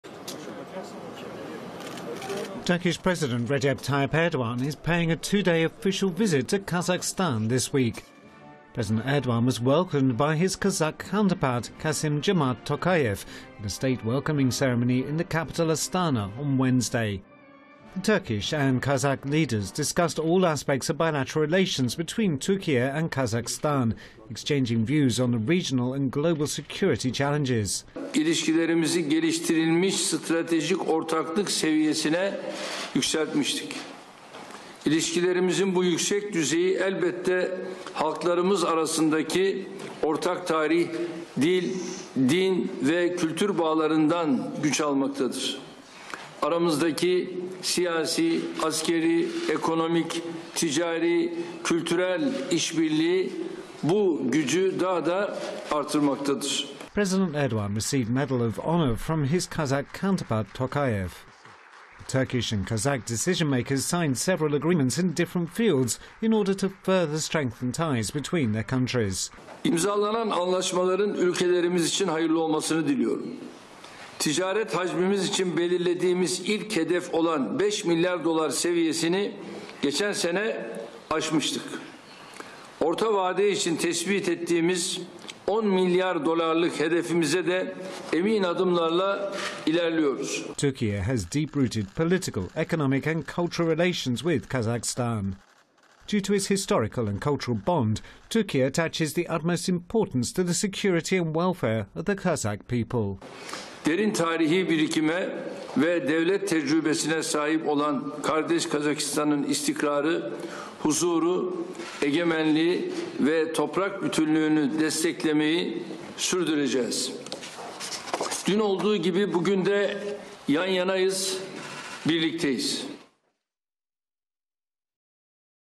Erdoğan, Tokayev discuss bilateral ties, int'l challenges
Turkish President Recep Tayyip Erdoğan and his Kazak counterpart Tokayev discussed bilateral ties and international challenges on Wednesday at a meeting in Kazakhstan.